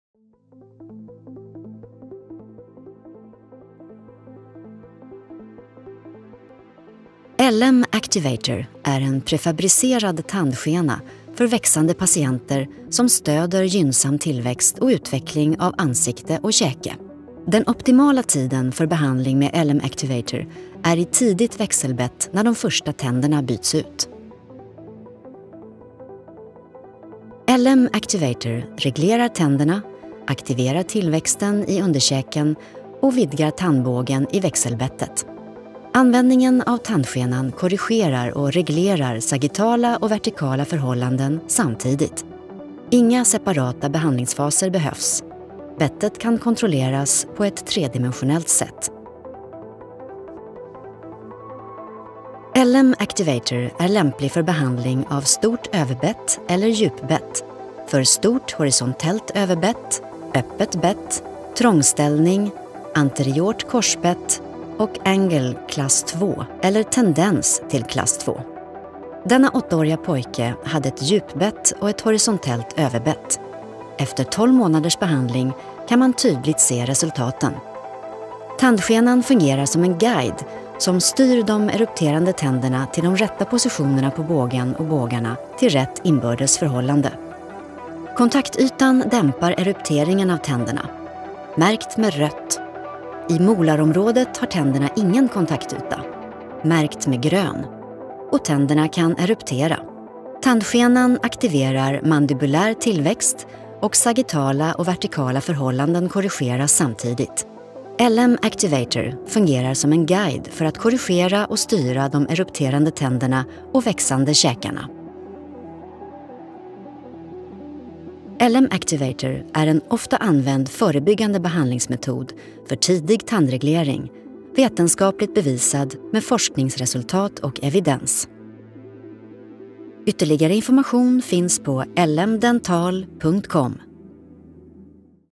Video Hur fungerar LM-Activator™ SV
LM-Activator™ AKTIVATOR OCH TANDSKENA AV SILIKON för växande patienter.

Manufacturer: LM-Dental™ │ LM-Instruments Oy │Norrbyn rantatie 8, FI-21600 Parainen │ Finland